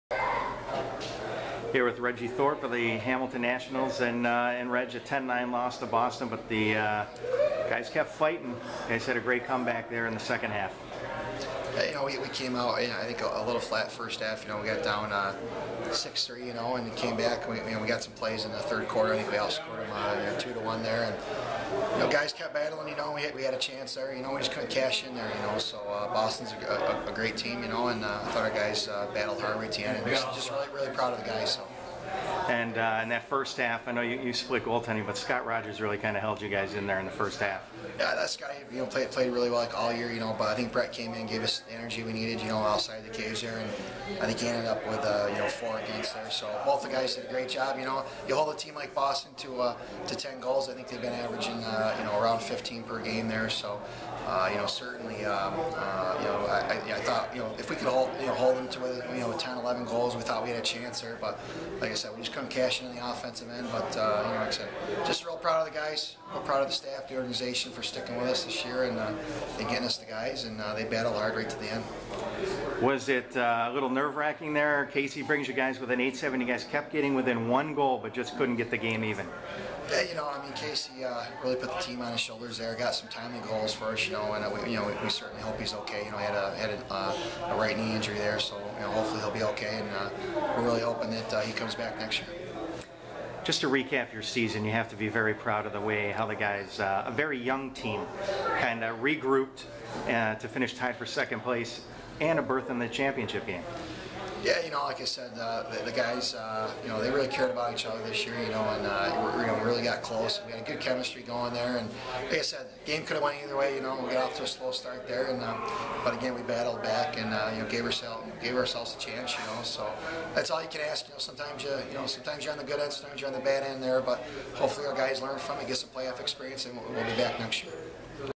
Former Knighthawk Regy Thorpe recaps Hamilton Nationals season (8-28-11)
Former Knighthawk Regy Thorpe recaps Hamilton Nationals' season. Thorpe completed hsi first season as the Nats head coach and led the team to the championship game.